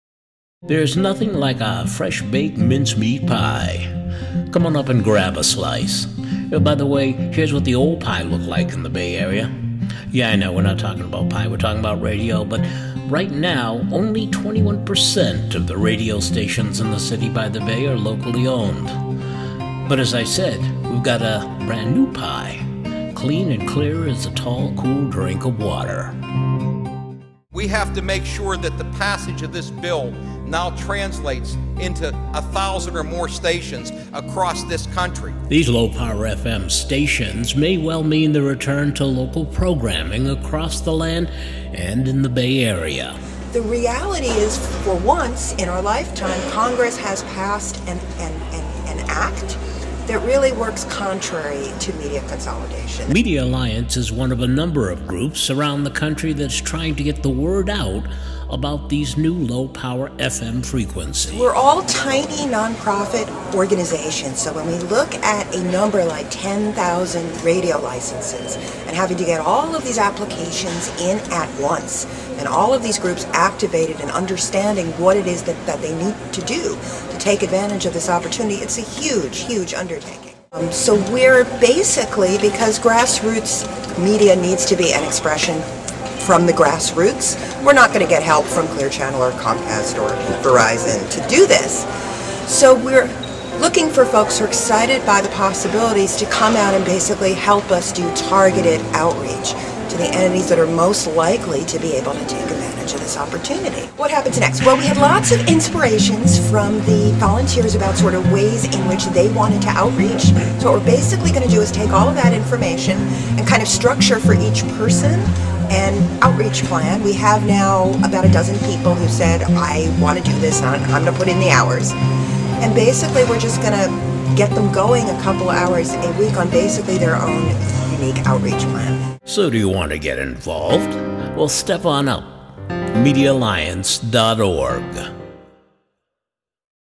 Low Power FM Media Alliance
A look at the emergence of new Low-Power FM radio licensing, and how you can get involved. Features an interview with Tracy Rosenberg Executive Director of Media-Alliance in the San Francisco Bay Area. The video was produced to the web by Media Alliance volunteer David Gilton Soma, of DialinSky.com.